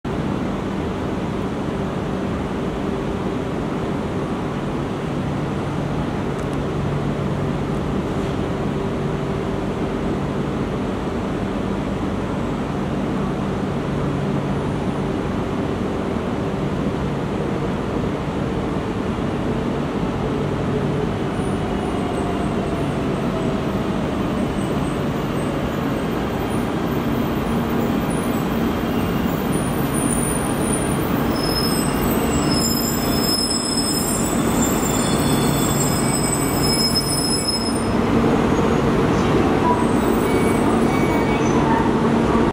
JR東日本上越新幹線E2系とき365号進站(新潟)
JR東日本上越新幹線E2系とき365号進站(新潟),攝於2018年10月27日,去日本玩~